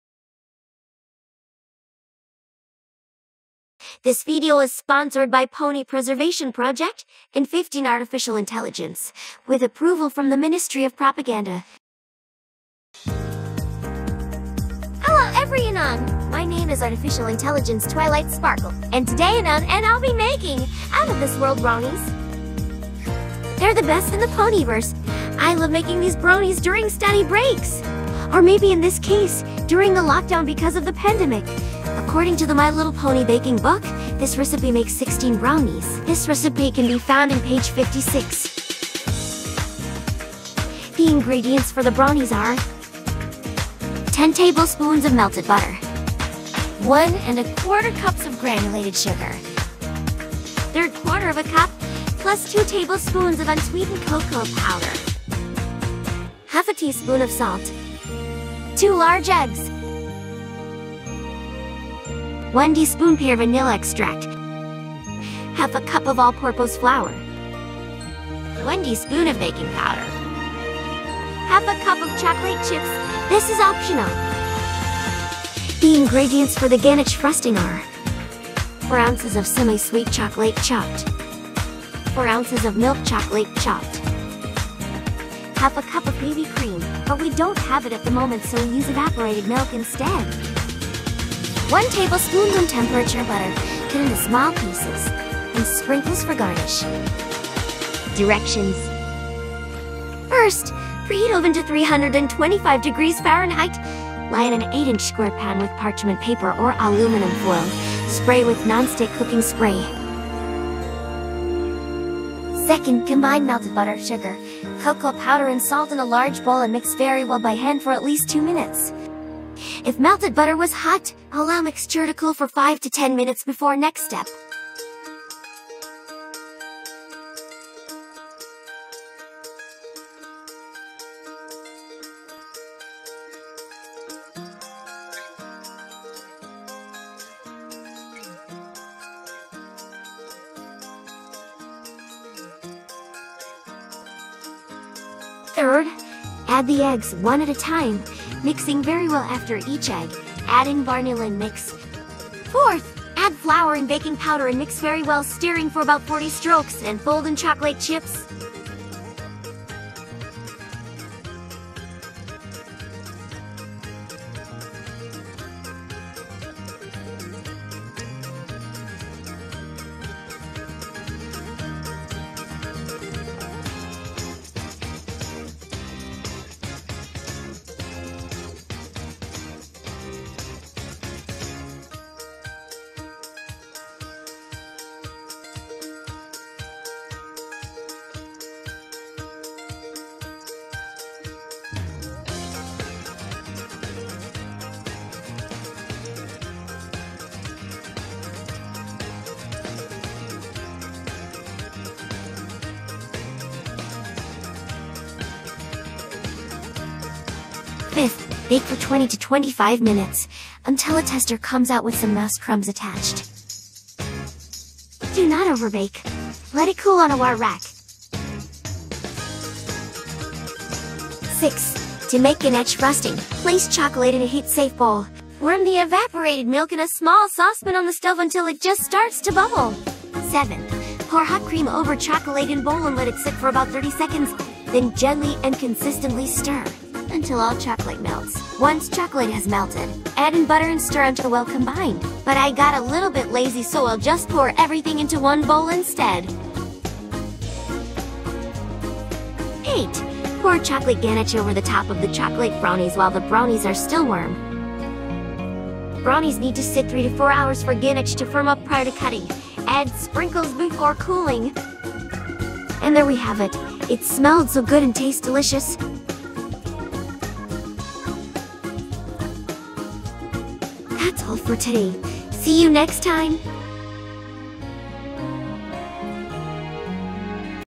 MLP Baking Book recipe: Out-of-this-World Brownies
video for /mlp/ /Baking/ Thread 35107430, No. 35108854 ^:)
thanks to PPP & 15.ai (that A.I. Twilight """Voice Actor""" in the video) for making this possible.